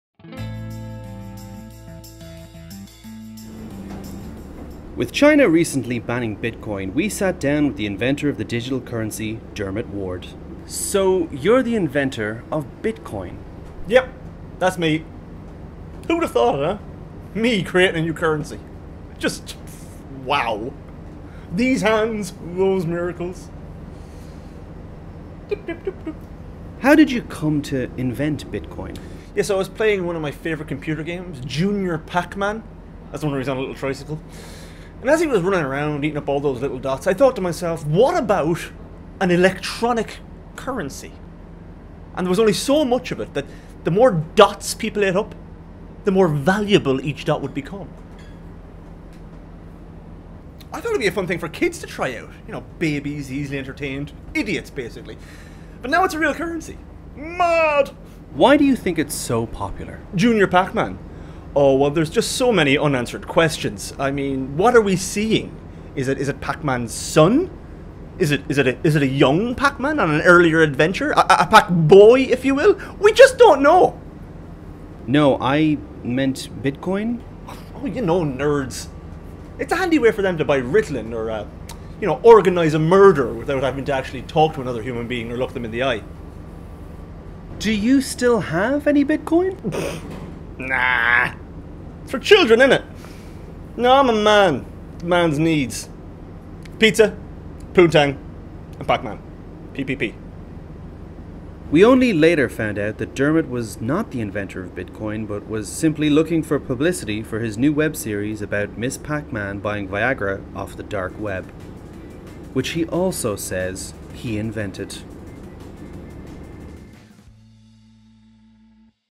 Mister Bitcoin | FOX & BLAKE
Everybody is talking about Bitcoin, but what is it?
To find out more, we spoke to it's inventor: Dermot Ward.